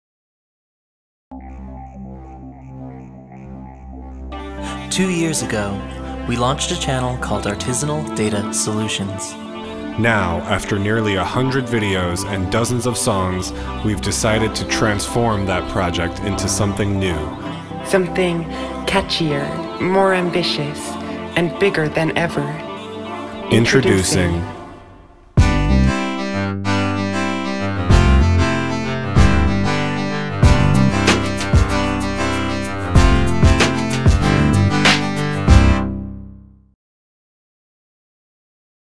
PHANTOM VIBRATIONS ANNOUNCEMENT
Announcing the end of Artisanal Data Solutions and the beginning of Phantom Vibrations.